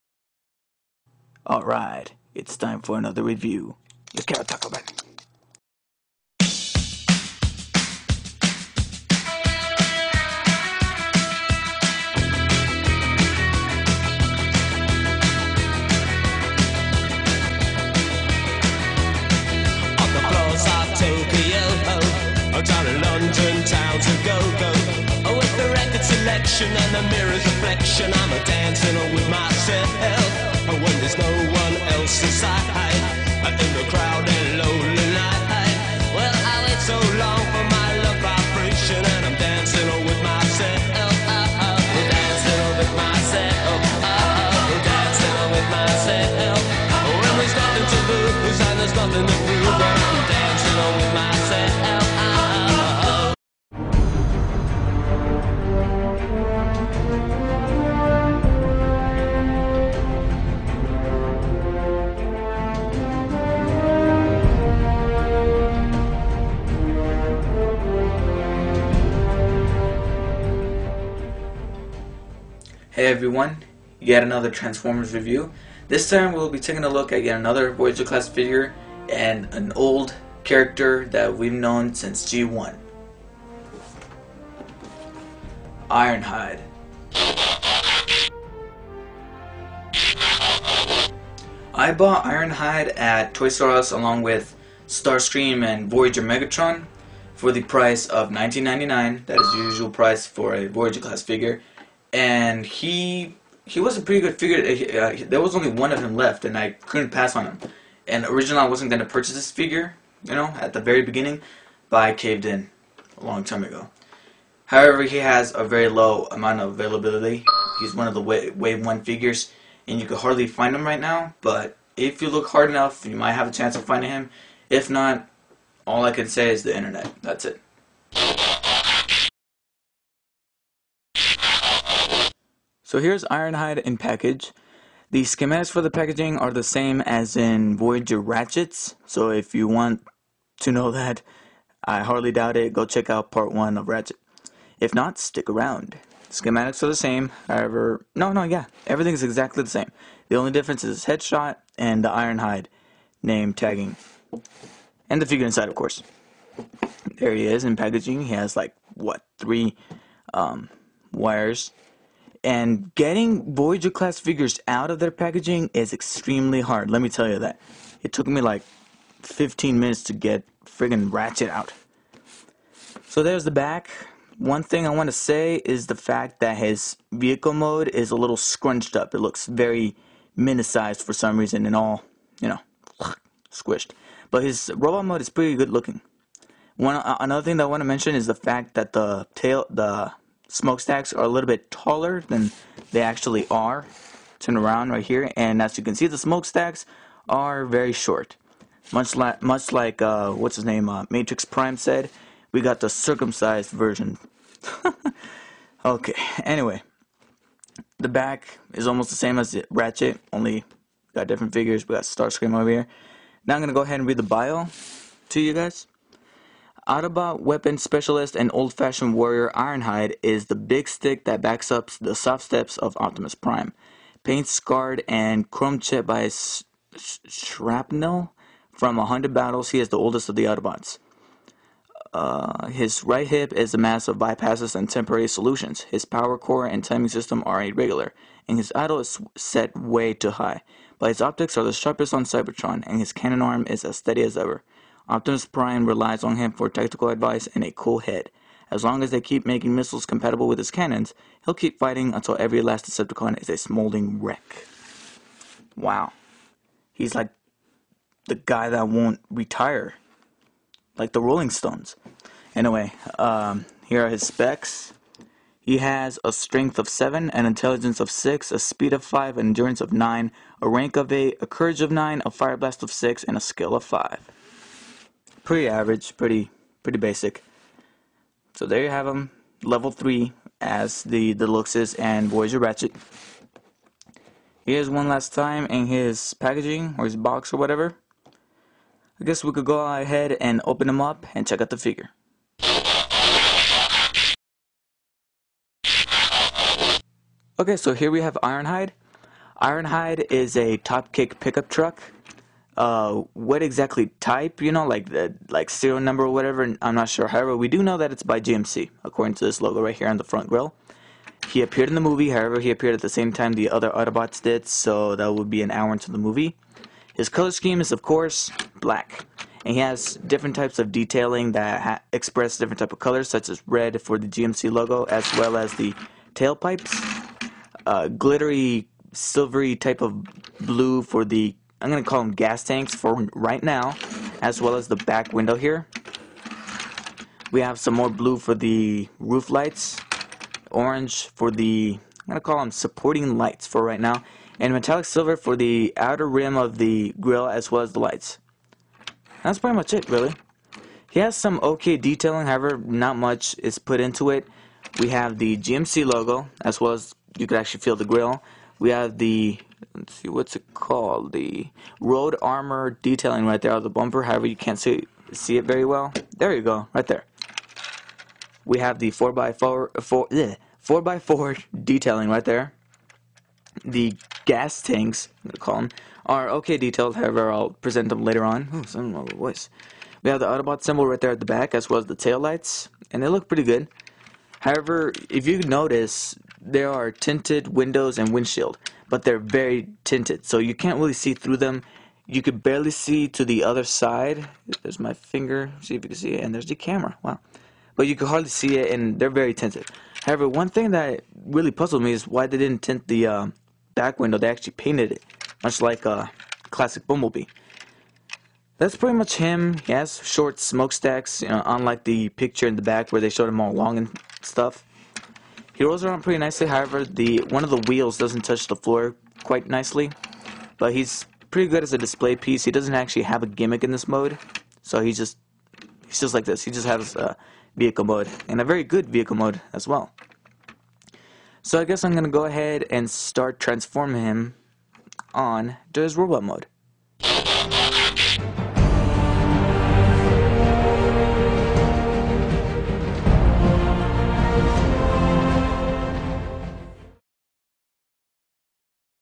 Blackspider David Video Reviews - Voyager pickup truck robot - Part 1
Old? More like authentic. In Part 1, I'll discuss:
- Overview
- Mint Condition
- Deep Analysis - Vehicle Mode